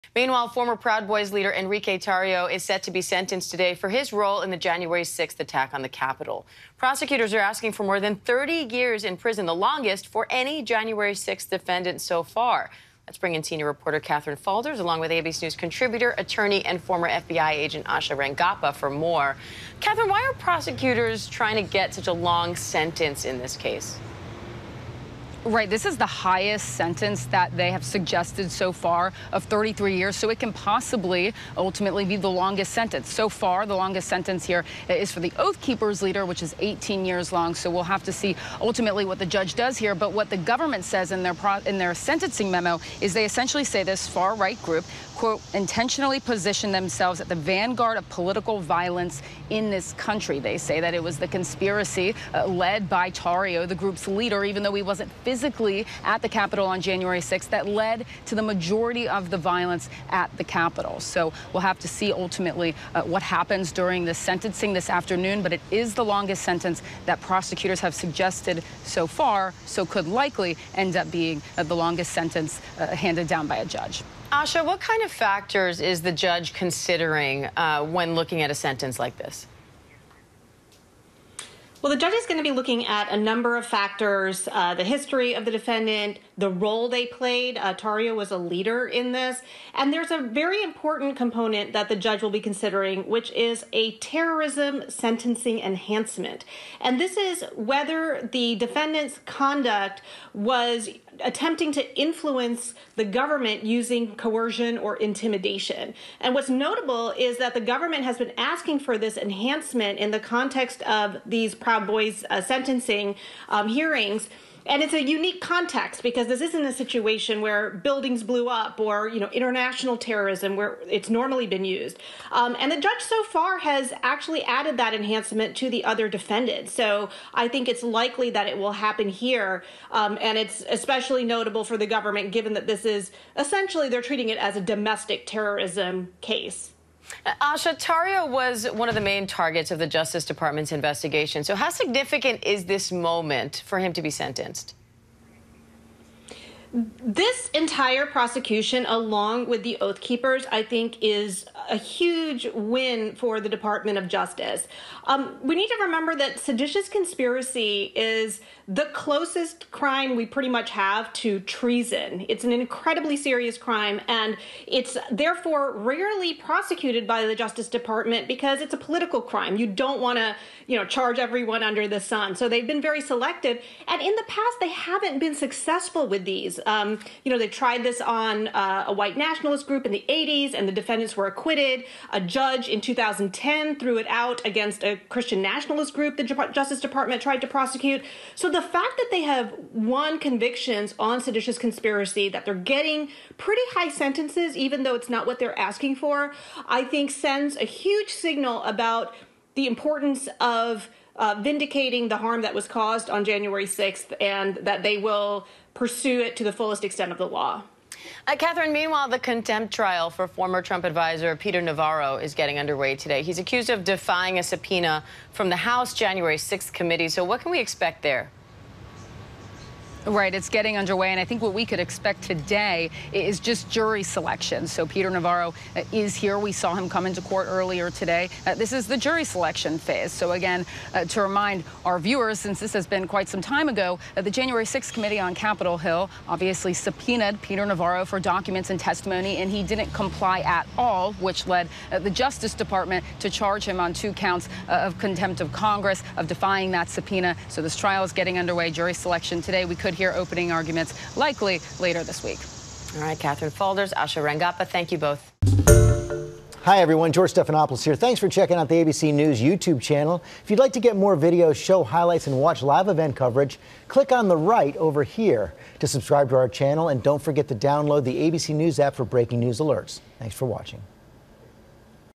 Former Proud Boys leader set to learn sentence after Jan. 6 attack
Prosecutors are asking for Enrique Tarrio to get more than 30 years in prison, the longest for any Jan. 6 defendant so far.